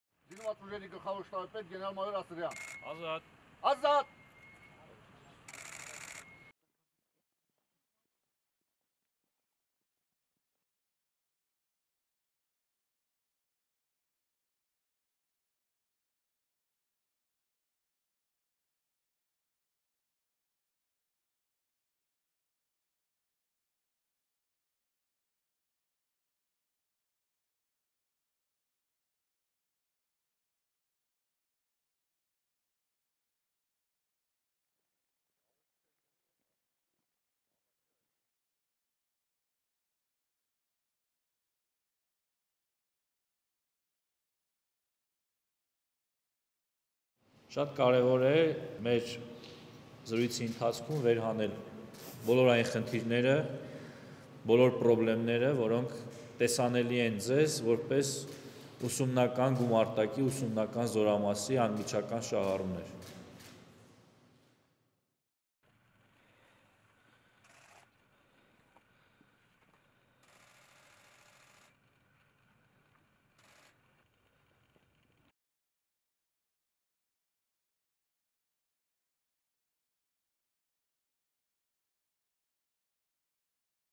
Սուրեն Պապիկյանն այցելել է N բանակային զորամիավորում
Դեկտեմբերի 2-ին ՀՀ պաշտպանության նախարար Սուրեն Պապիկյանը՝ ԶՈՒ ԳՇ պետ, պաշտպանության նախարարի առաջին տեղակալ, գեներալ-մայոր Էդվարդ Ասրյանի և Լոռու մարզպետ Արամ Խաչատրյանի ուղեկցությամբ, այցելել է N բանակային զորամիավորում:
Զորամասերից մեկում պաշտպանական գերատեսչության ղեկավարը հանդիպել է հատուկ նշանակության և դեսանտագրոհային ստորաբաժանումների զինծառայողների հետ, հետաքրքրվել ծառայության կազմակերպման ընթացքով, առկա խնդիրներով, անձնակազմի հանդերձավորման և հաստիքային սպառազինության հետ կապված հարցերով:
Այնուհետև Սուրեն Պապիկյանը և գեներալ-մայոր Էդվարդ Ասրյանը հանդիպել ու զրուցել են ուսումնական գումարտակի ժամկետային զինծառայողների հետ, հաջողություններ մաղթել առաջիկա ավարտական քննությունների կապակցությամբ:
Պաշտպանության նախարարը պատասխանել է զինվորներին հետաքրքրող հարցերին, ներկայացրել ԶՈւ-ում մեկնարկած բարեփոխումների ընթացքը:
Պաշտպանության նախարարը և ԶՈւ ԳՇ պետ, պաշտպանության նախարարի 1-ին տեղակալը հարգանքի տուրք են մատուցել 2020 թվականի պատերազմում անմահացած հերոսների հիշատակին:
Այցի ավարտին նախարար Պապիկյանը ծանոթացել է զորամասի տարածքում կառուցվող նոր զորանոցի շինարարական աշխատանքների ընթացքին: